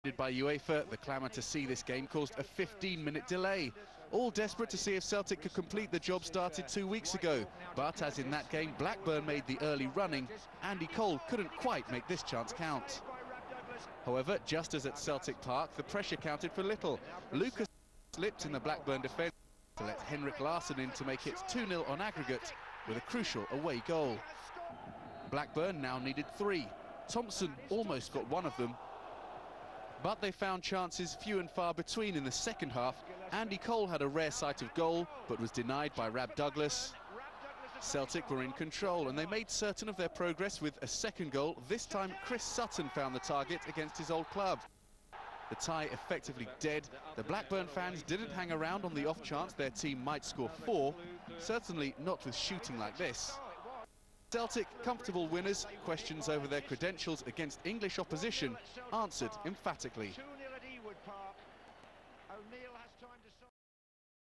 Blackburn Rovers v Celtic - UEFA Cup - 14/11/2002
Match played at Ewood Park on November 14th, 2002.